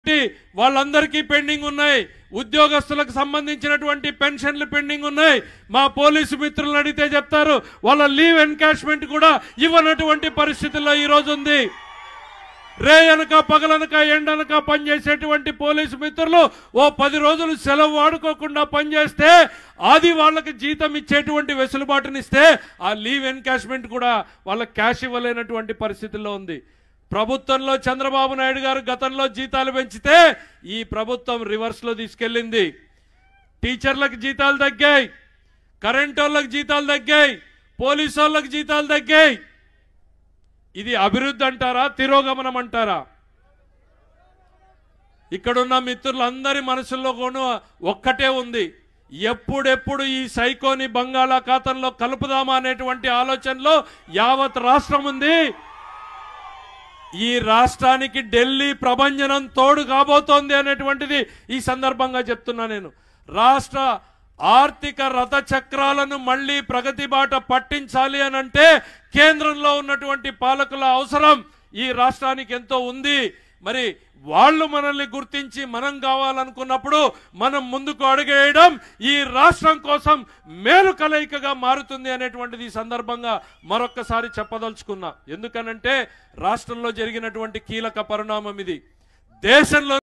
Payyavula Keshav Open Challenge to YS Jagan | మగాడిలా ఛాలంజ్ చేస్తున్న .. నిలబడే దమ్ముందా జగన్ ..?
Payyavula Keshav Open Challenge to YS Jagan | మగాడిలా ఛాలంజ్ చేస్తున్న .. నిలబడే దమ్ముందా  జగన్ ..? | Focus News Telugu